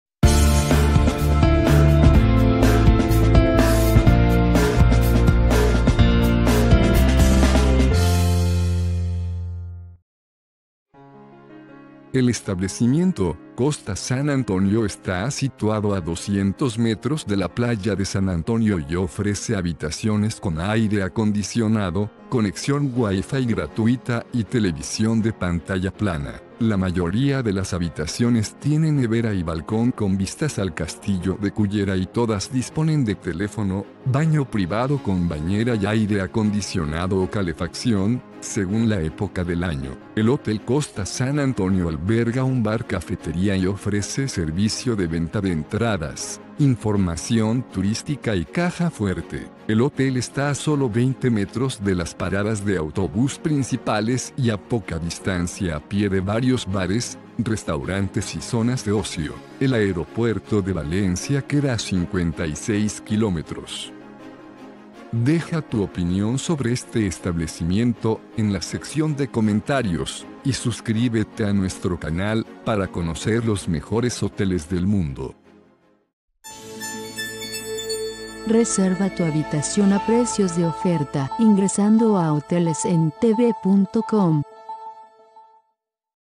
Hotel Costa San Antonio en Cullera
Hotel Costa San Antonio ofrece 18 habitaciones con bañera, teléfono y aire acondicionado. La edificación de Hotel Costa San Antonio se encuentra localizada en una encantadora región de Cullera. Los viajeros de Hotel Costa San Antonio le otorgaron una calificación de 8.2.